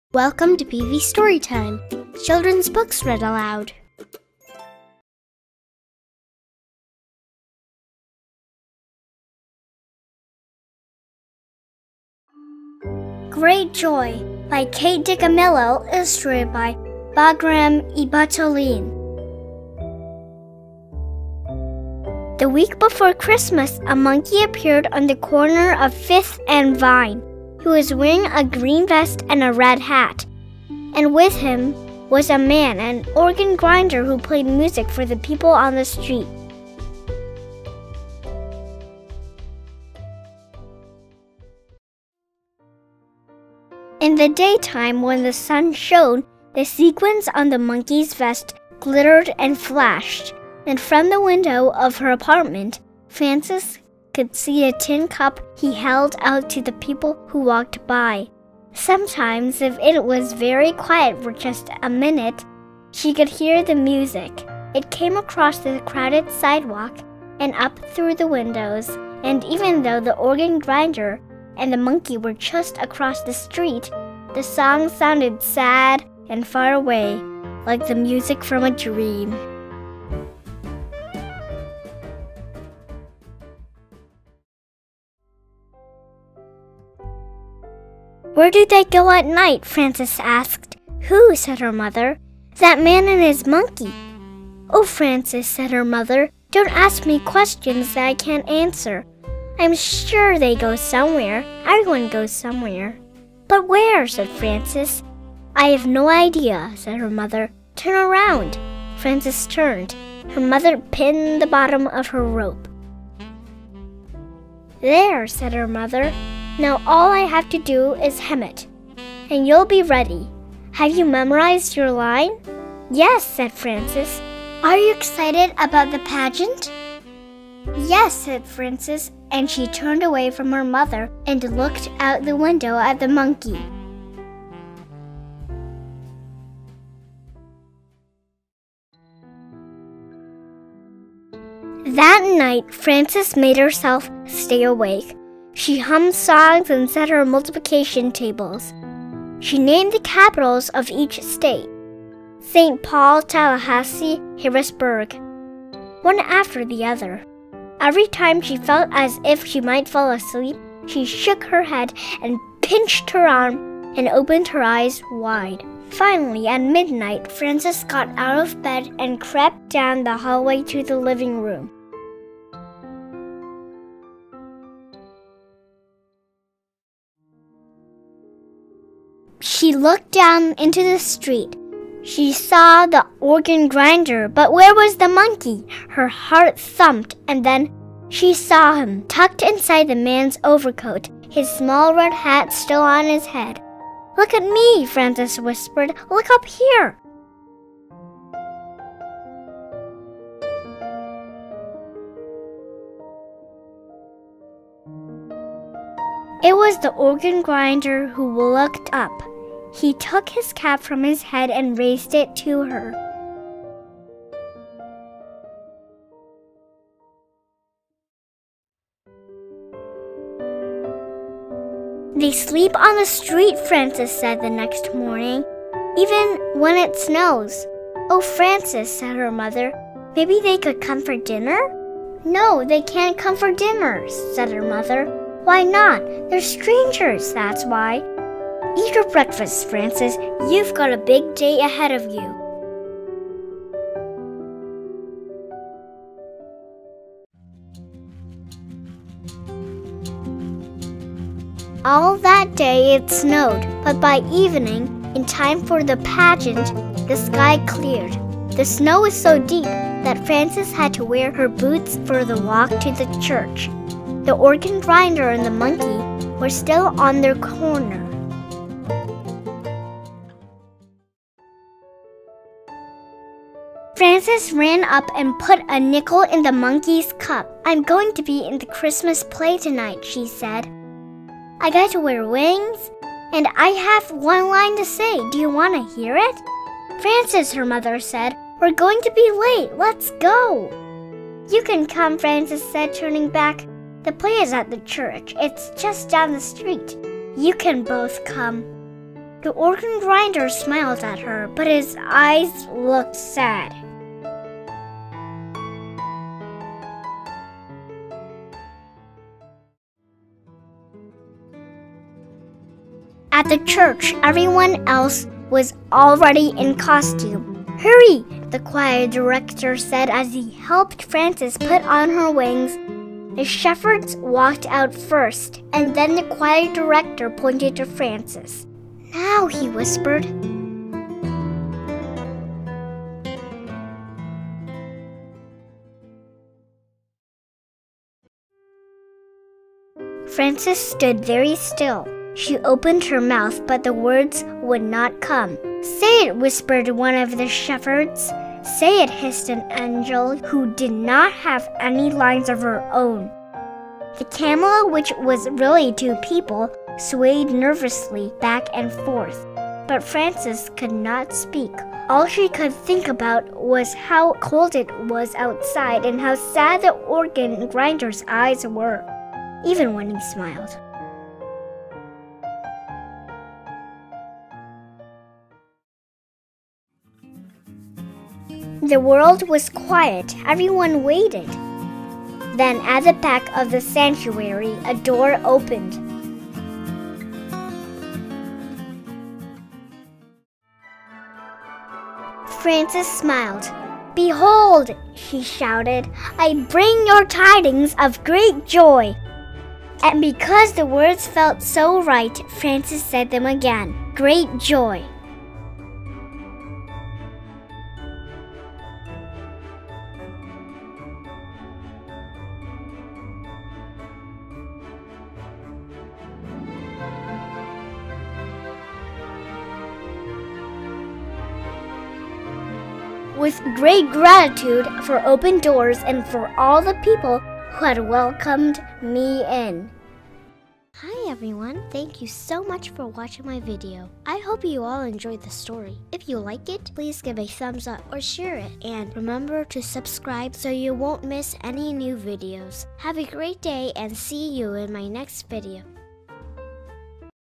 ⛪ 🔔👼 Christmas Books For Kids - GREAT JOY by Kate DiCamillo and Bagram Ibatouline
Deep in the purple dusk of winter, a monkey and an organ grinder appear on the corner of Fifth and Vine. Frances watches the monkey hole out his silver cup to the busy passersby. Sometimes, when the traffic is very quiet for just a minute, she can hear the organ’s sweet, sad music.
But Frances is troubled when she notices that the man and the monkey are still on the street late at night. Where do they sleep? She can’t stop thinking about them, even while she’s practicing her line and getting her costume ready for the local Christmas pageant. When the moment finally comes for Frances to speak, the gift of compassion allows her to share the perfect words with everyone at just the right time.
In this, her first picture book, Newbery Medalist Kate DiCamillo again pairs with artist Bagram Ibatouline to share a luminous holiday story about the miraculous, powerful secret of joy.

Children's books read aloud
PV storytime